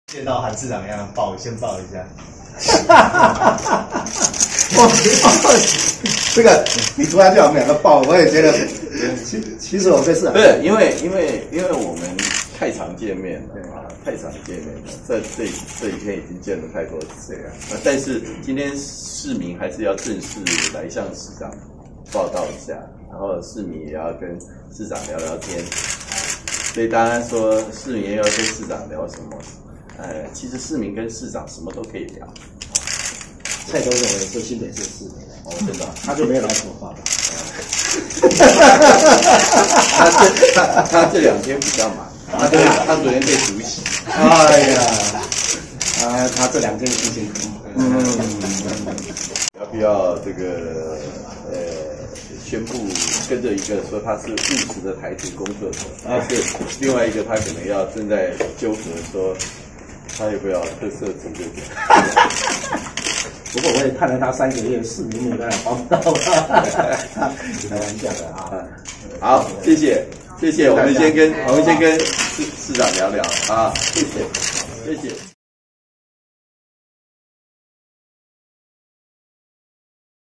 「回娘家」拜會侯友宜 朱立倫：以市民身分向市長報告
投入國民黨2020總統初選的前新北市長朱立倫，今天上午「回娘家」，至新北市政府拜會市長侯友宜，兩人一見面就閒話家常，朱立倫說：「我今天是以市民的身分向市長報告。」侯友宜也開玩笑說：「蔡總統也是新北市民，她也沒找我報告。」引起現場哄堂大笑。